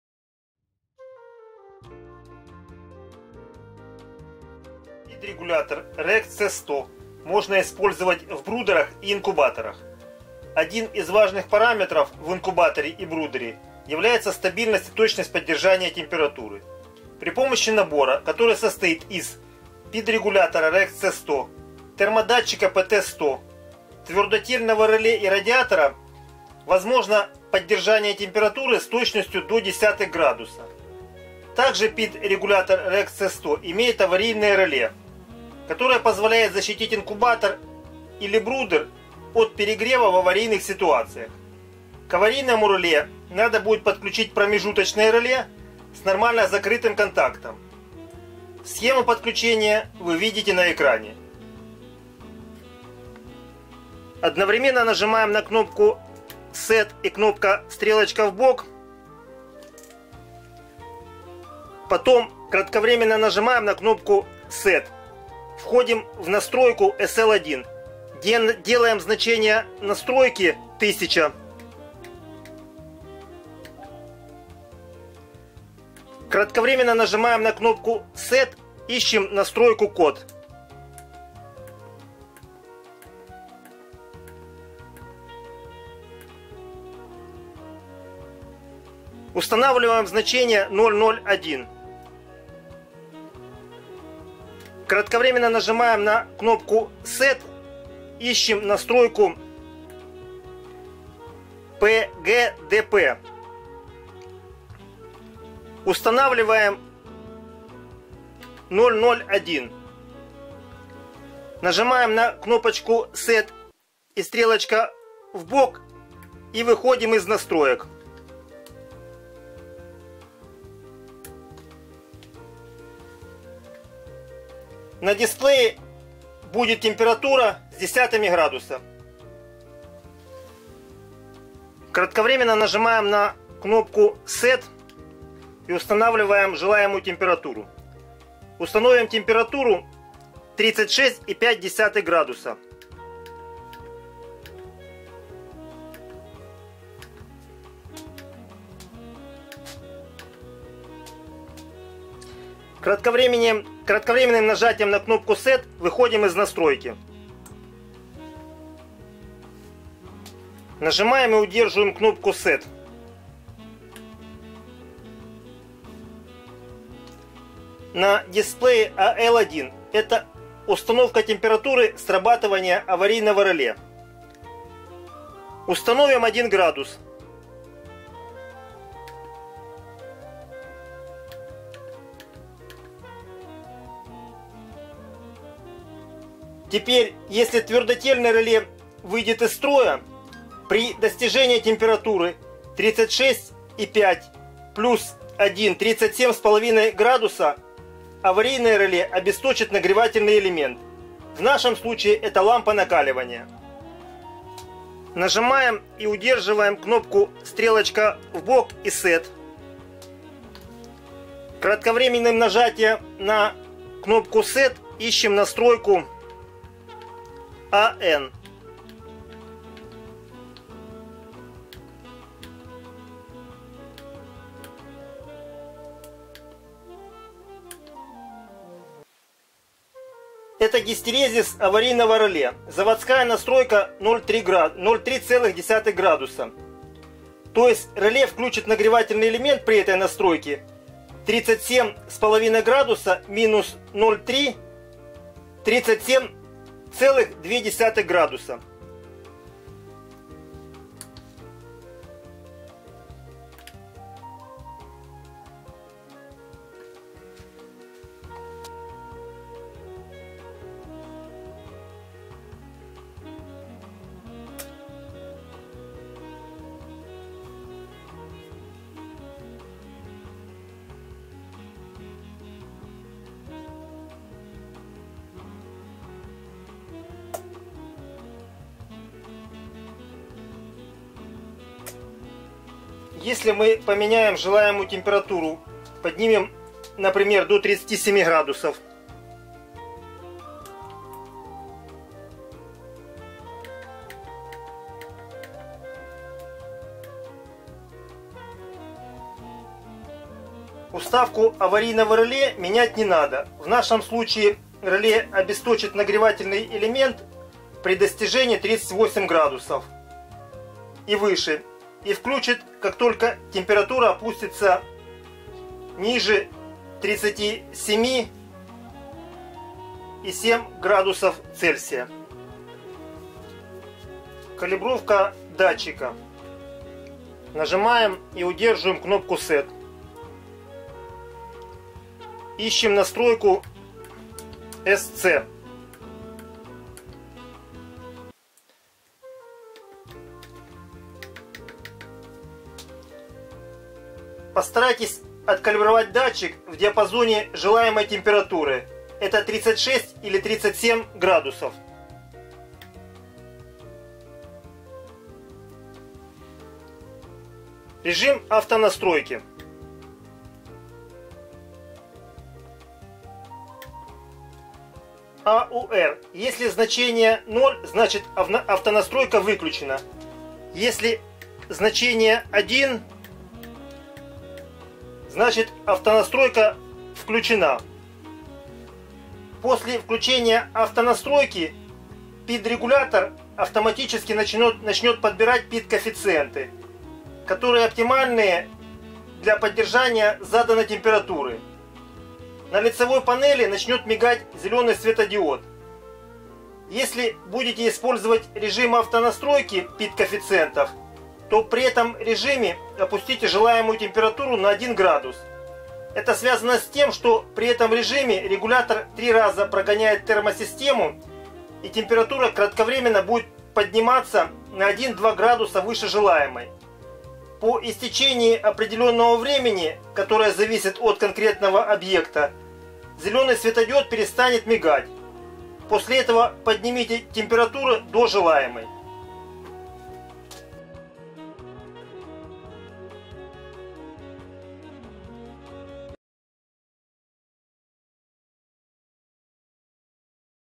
Настройка PID регулятора REX C100 для брудеров и инкубаторов. REX C100 dlya bruderov i inkubatorov
В этом видео мы покажем, как настроить PID регулятор REX C100 для брудеров и инкубаторов. Один из важных параметров в инкубаторе и брудере является стабильность и точность поддержания температуры.
При помощи набора, который состоит из
PID регулятора РЕХ С100
Термодатчика РТ 100
Твердотельного реле и радиатора.
Схема подключения : 00:50
Возможно поддержание температуры с точностью до десятых градуса.
Также PID регулятор РЕХ С100 имеет аварийное реле, которое позволяет защитить инкубатор или брудер от перегрева в аварийных ситуациях.
К аварийному реле надо будет подключить промежуточное реле с нормально открытым контактом.
Пид-терморегуляторы rex-c100 :
Видео по настройке других безкорпусных терморегуляторов